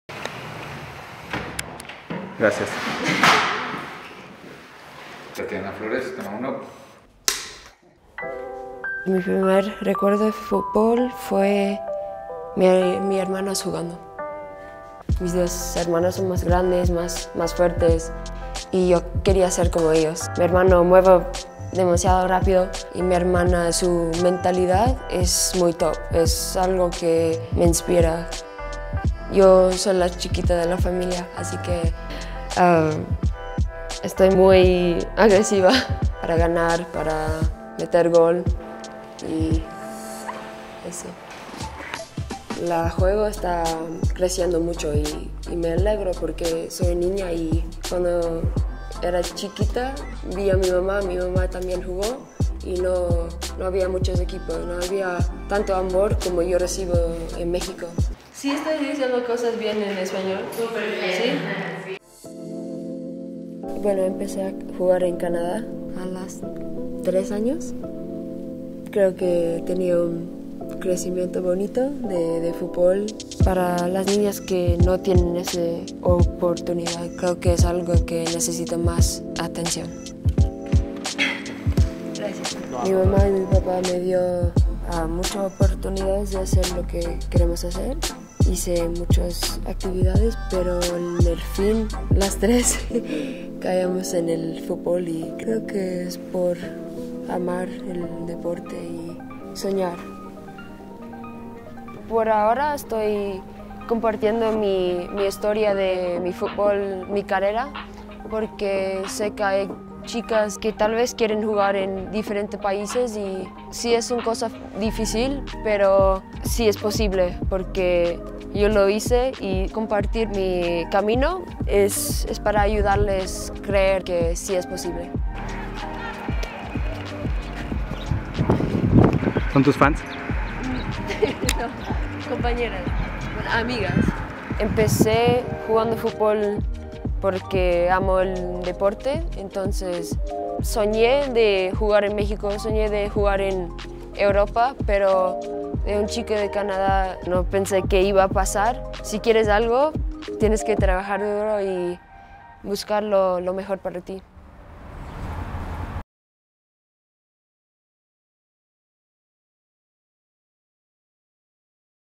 Girl Power | 𝐓𝐚𝐭𝐢𝐚𝐧𝐚 𝐅𝐥𝐨𝐫𝐞𝐬
GirlPower regresa y lo hace a lo grande con la seleccionada mexicana y delantera del Real Oviedo, 𝐓𝐚𝐭𝐢𝐚𝐧𝐚 𝐅𝐥𝐨𝐫𝐞𝐬. Entrevista exclusiva de adidas para Culto Fútbol.